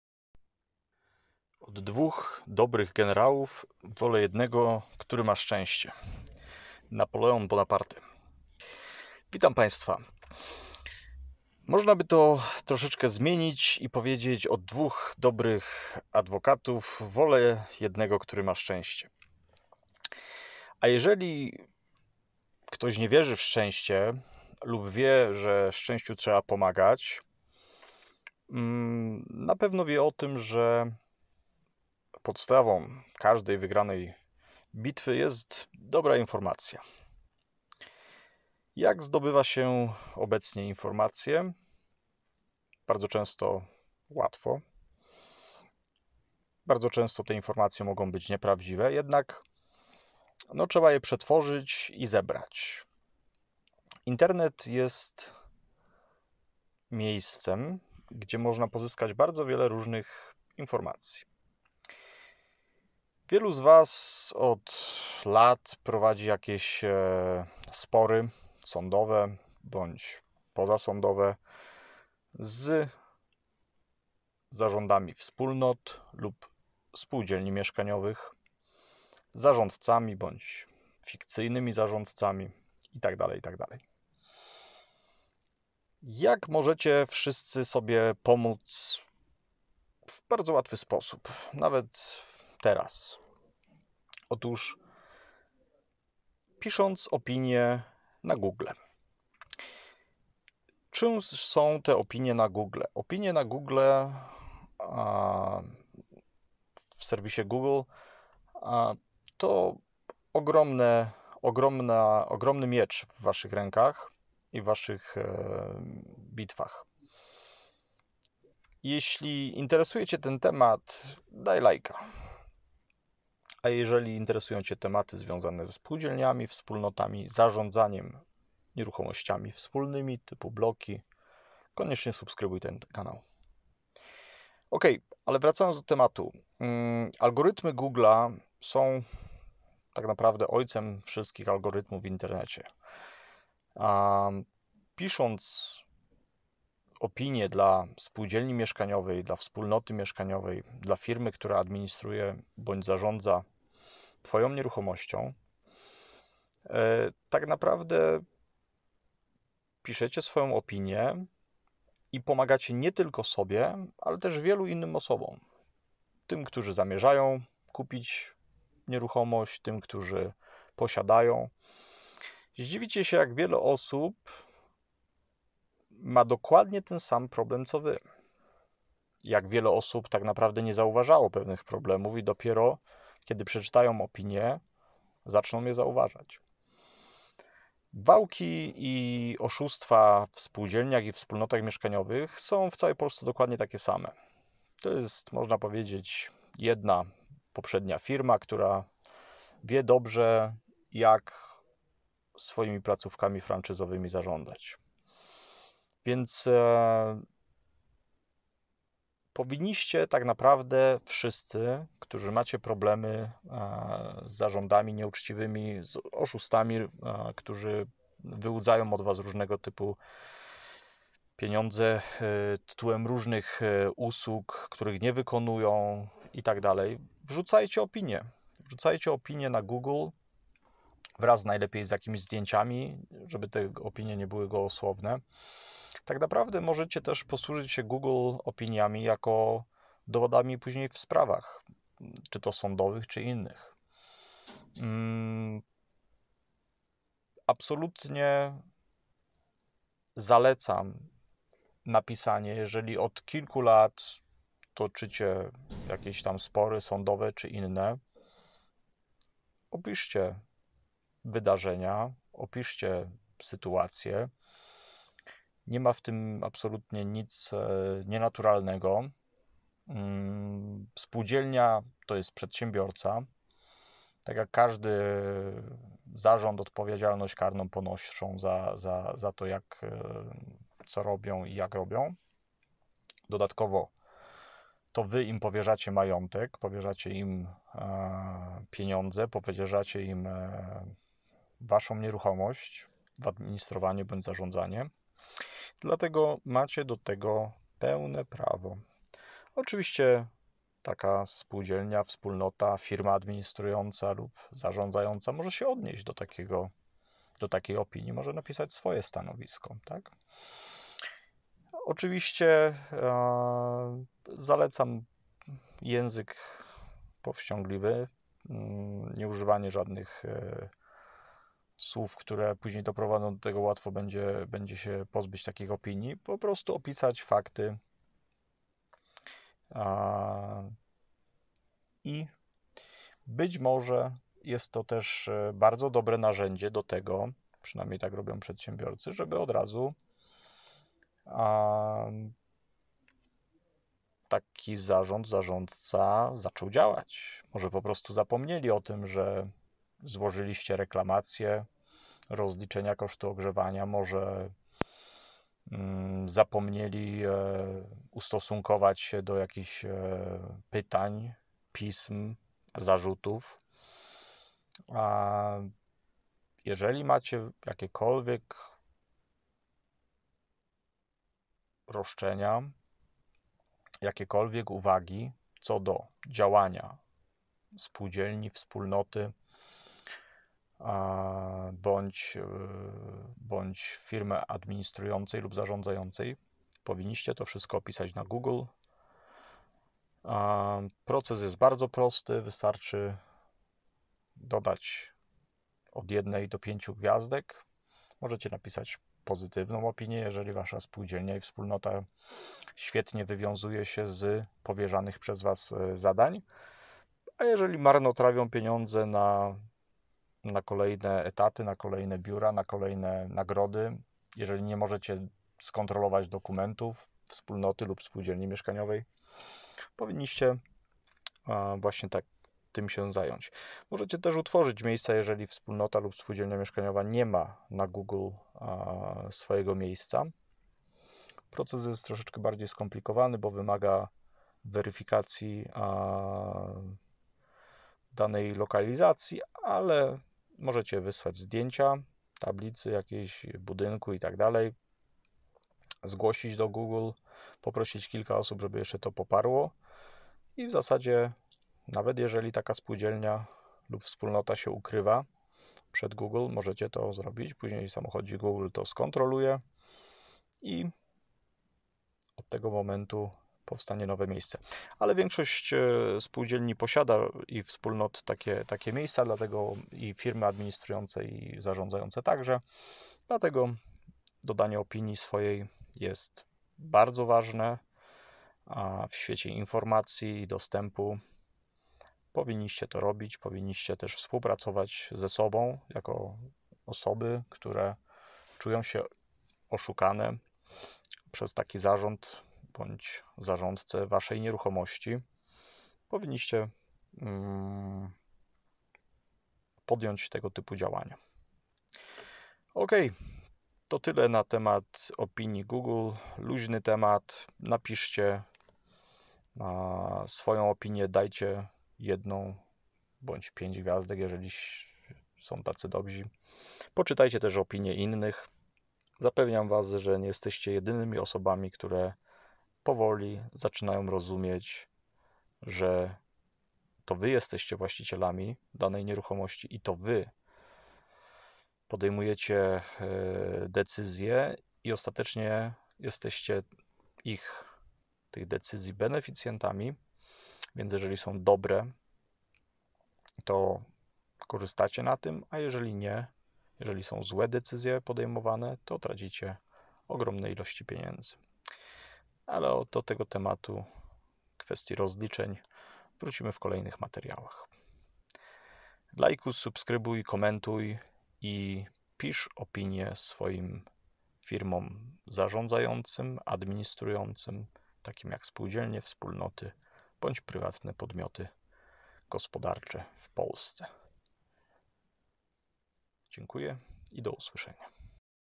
Napisz opinię google
Spółdzielnia, wspólnota, firma administrująca lub zarządzająca to przedsiębiorcy. Działają dla Was, klientów. Masz prawo napisać opinię google i ocenić ich pracę. To bardzo proste:
1. Znajdź spółdzielnię, wspólnotę lub firmę administrującą, zarządzającą na google maps
2. Przejdź do zakładki opinie
3. Zaznacz ilość gwiazdek- oznaczającą Twoje zadowolenie lub brak zadowolenia.
4. Napisz kilka słów.
5. Po pewnym czasie i zatwierdzeniu, opinia będzie widoczna dla wszystkich.
6. Wykorzystaj opinie google w sprawach sądowych jako dowód niezadowolenia wielu osób, na sprawowanie zarządu nad Twoją nieruchomością.

@Nieruchomości w Polsce- to musisz wiedzieć